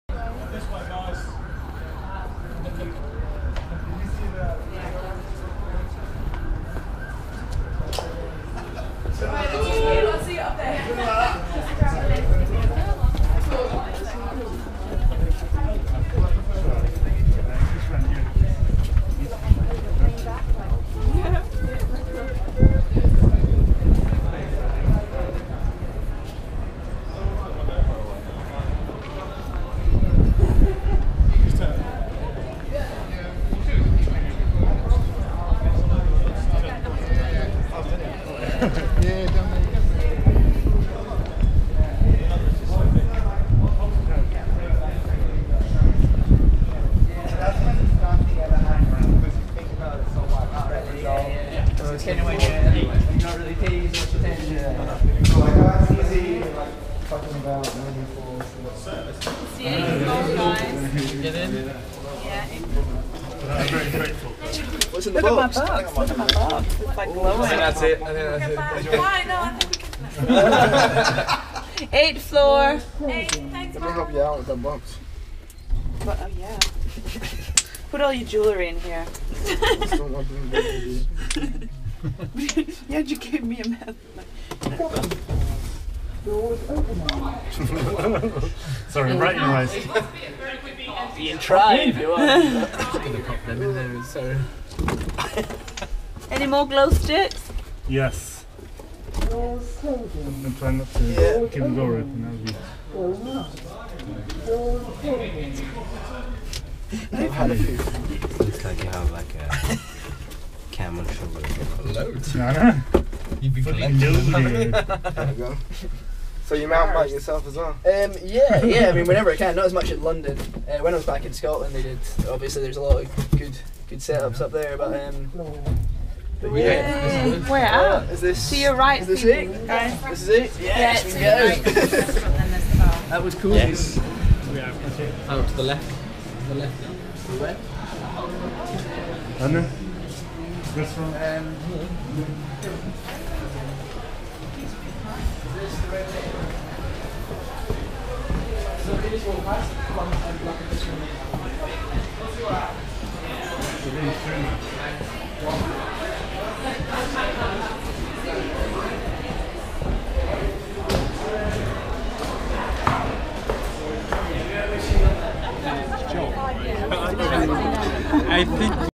GoPro Hero4 Silver 1080p video footage
Testing the new GoPro Hero4 Silver. Shooting at 720p as part of a preview day before the official launch. This video is to show how the camera copes with various light sources at night.